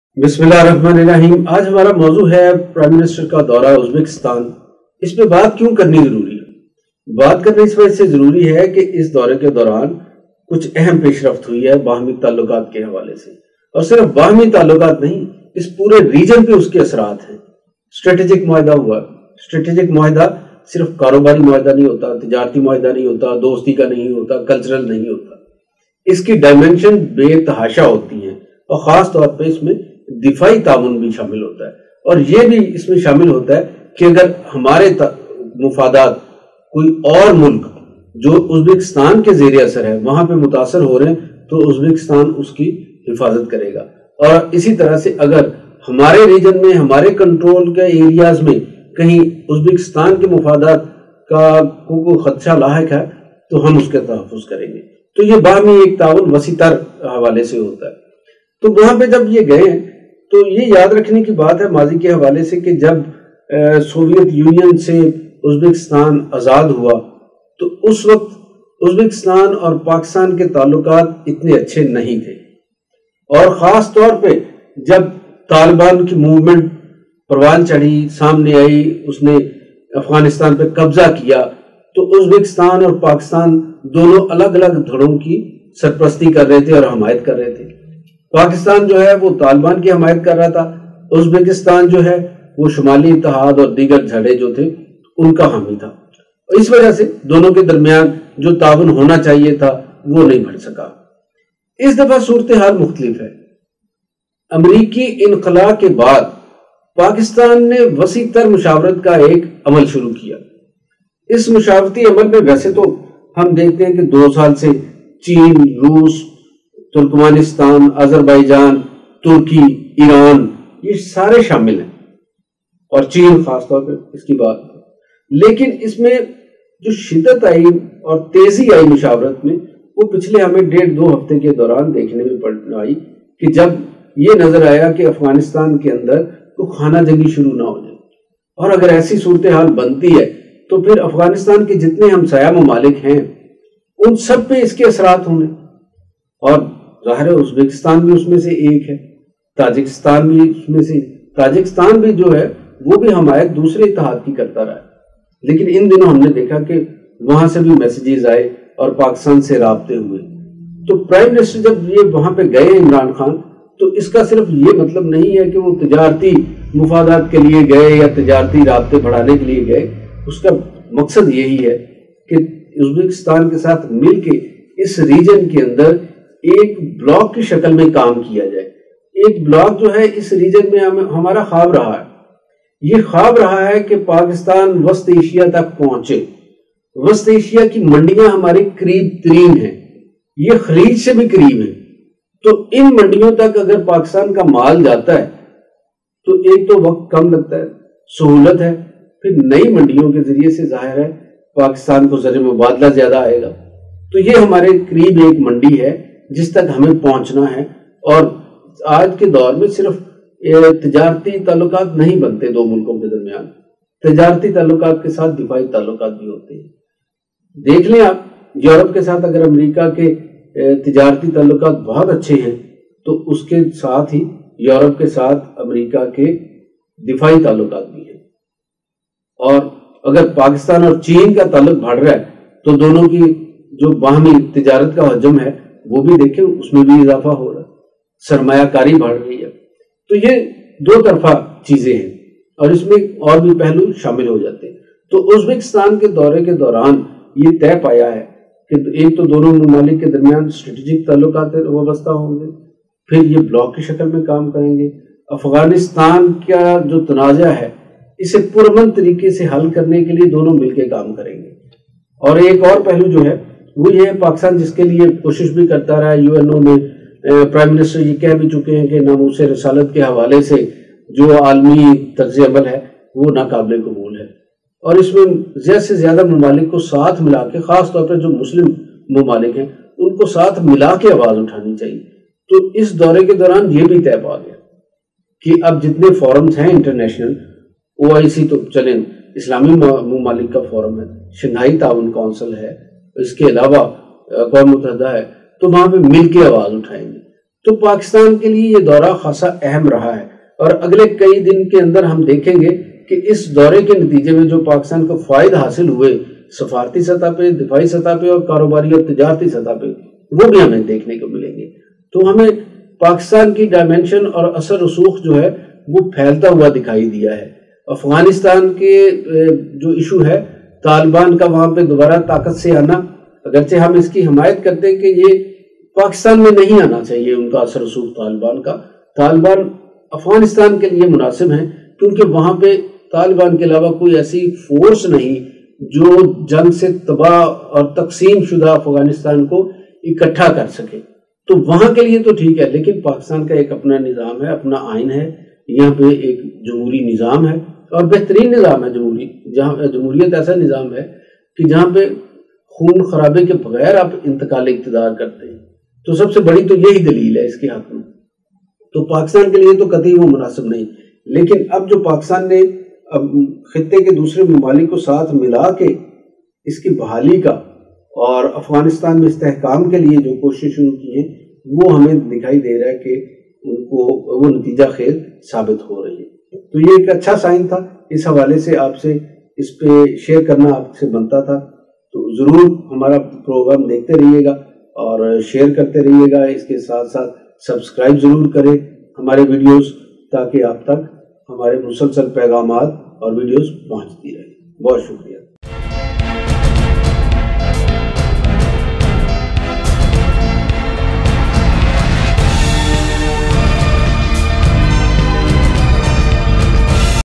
PM Imran Khan Historic Welcome in Uzbekistan | Exclusive Video
PM Imran Khan Dabbang speech at business forum in Uzbekistan |
PM Imran Khan Historic Royal Welcome in Uzbekistan | Grand Reception